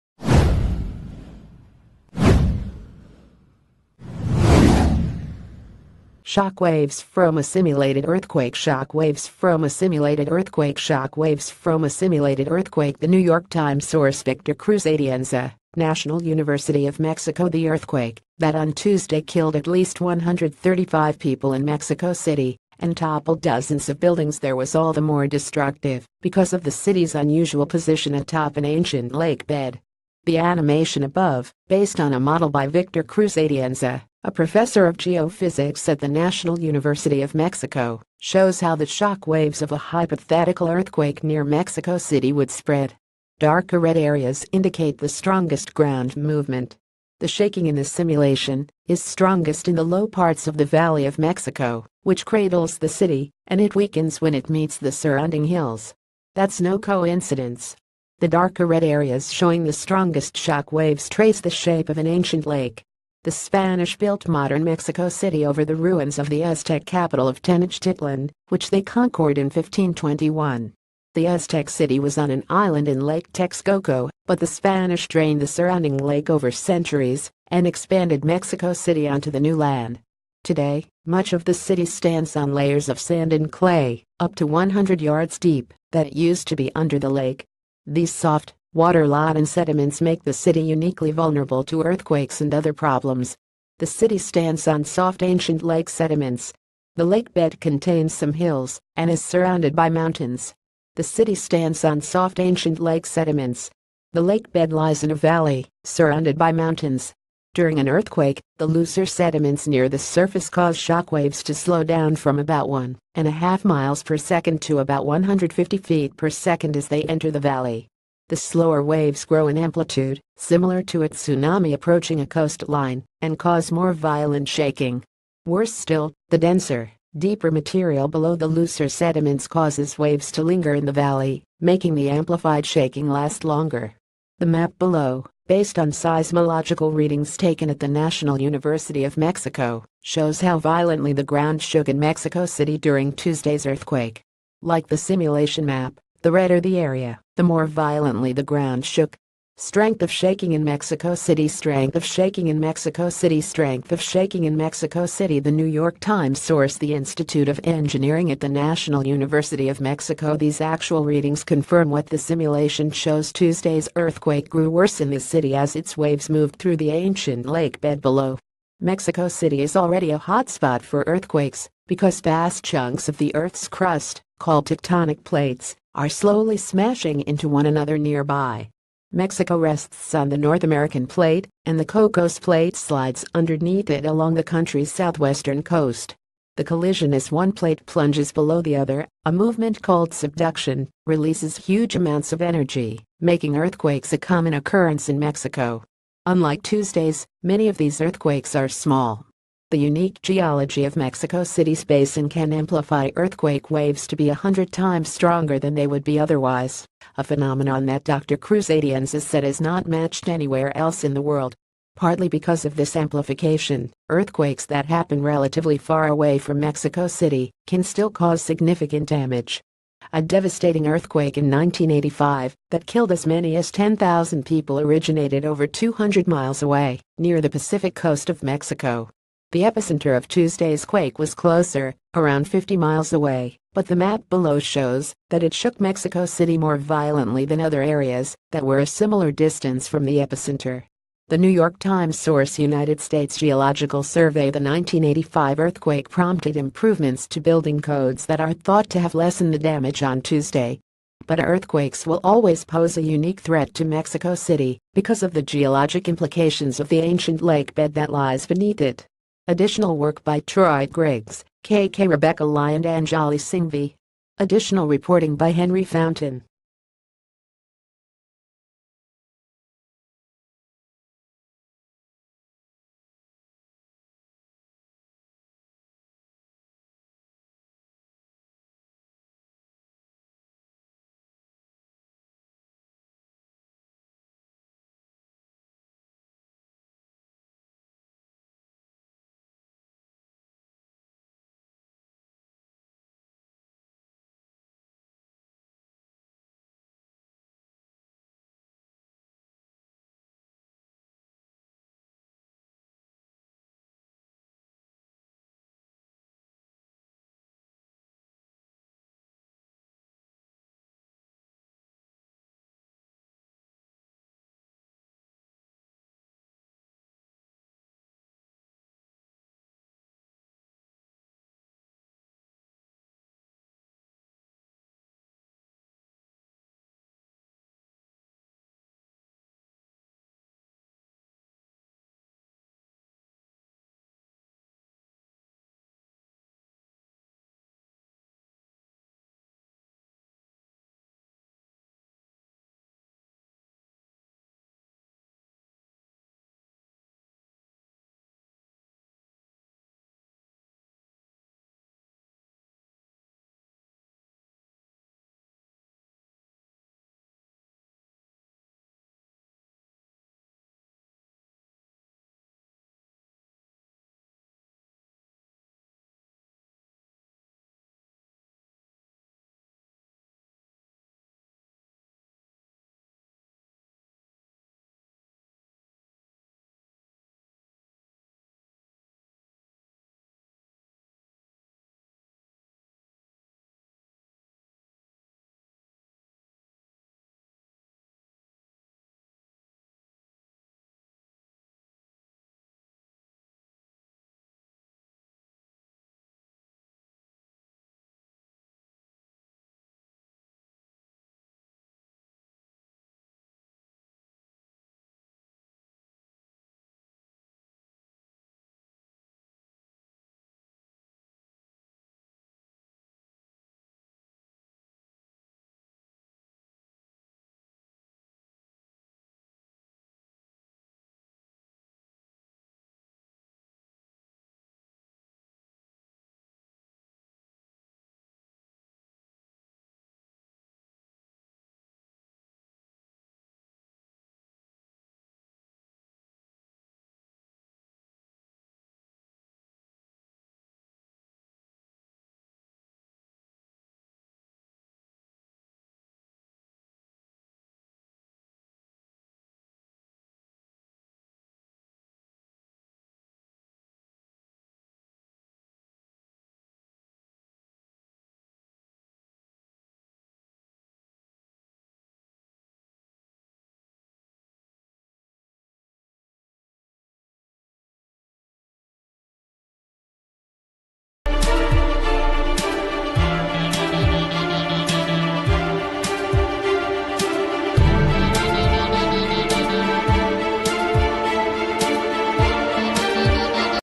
Breaking News | Mexico city was built on an ancient lake bed. that makes earthquakes much worse.
Mexico city was built on an ancient lake bed. that makes earthquakes much worse.

Shock waves froma simulated earthquake Shock waves froma simulated earthquake Shock waves froma simu...